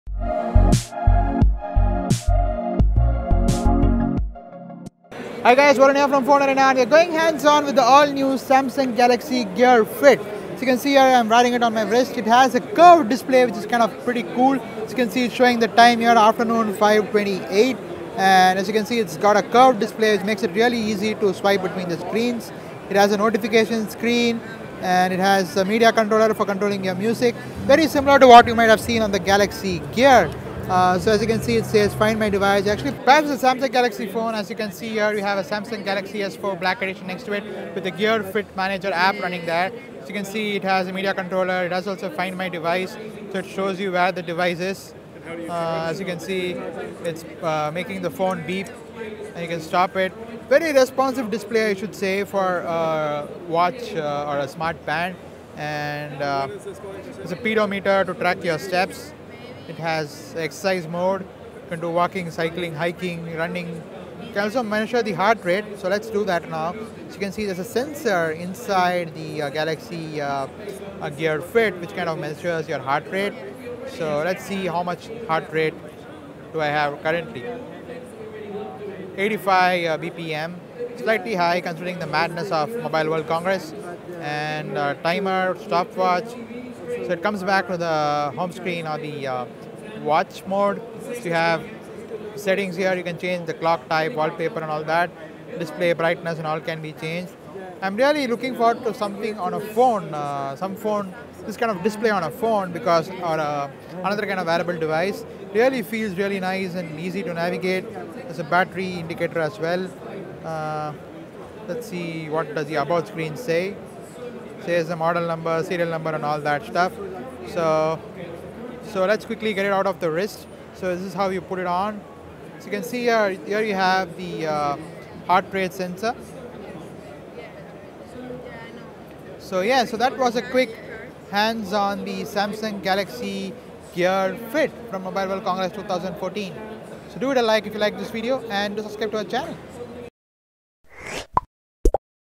Samsung Gear Fit Hands On
Samsung Gear Fit Hands On by FoneArena.
takes a hands on look at the newly launched Samsung Gear Fit at MWC 2014.